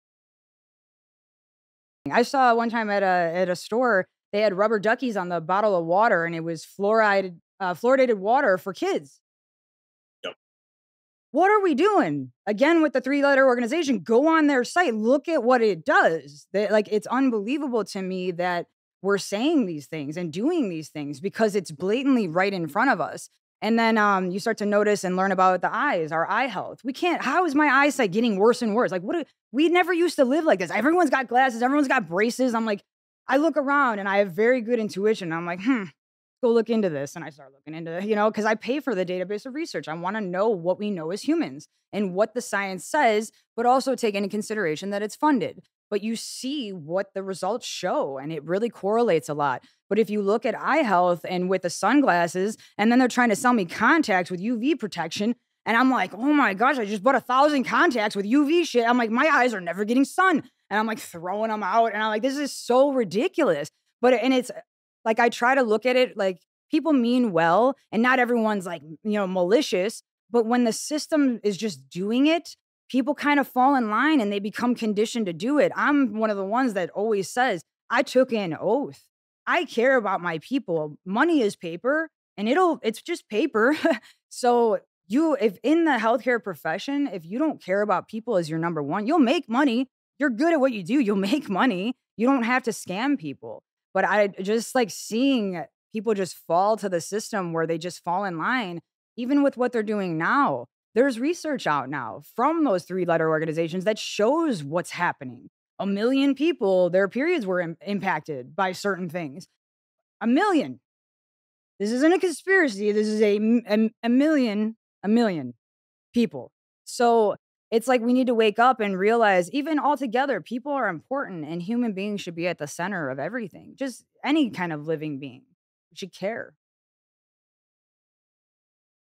What Are the Motives Behind These 3 Letter Organizations
Check out the full episode with Matt Roeske and Dr. Jena Gatses